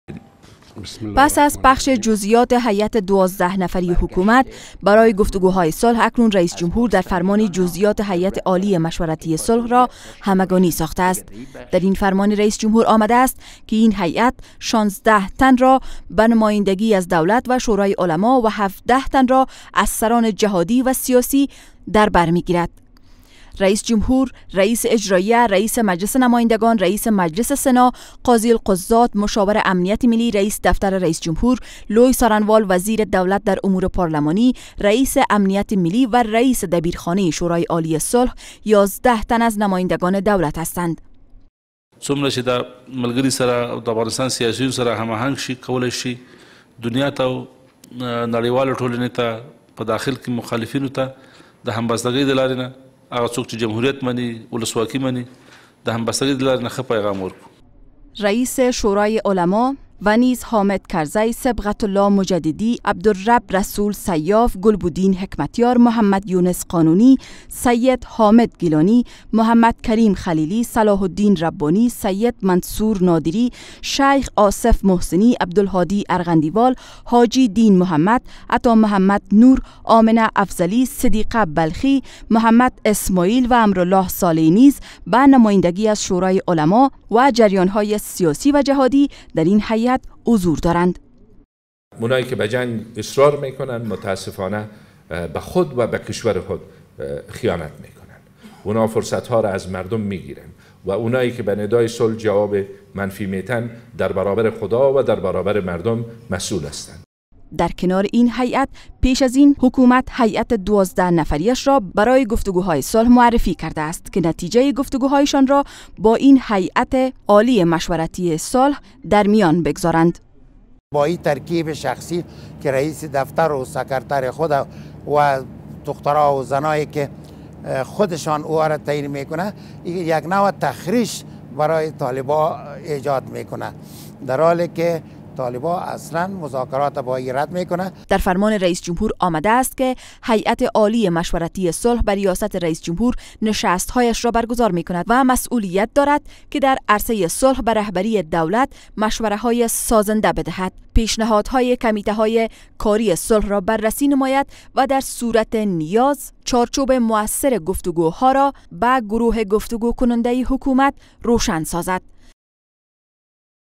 Govt Announces High Advisory Board For Peace
ایجاد بورد عالی مشورتی صلح و تیم مذاکره کننده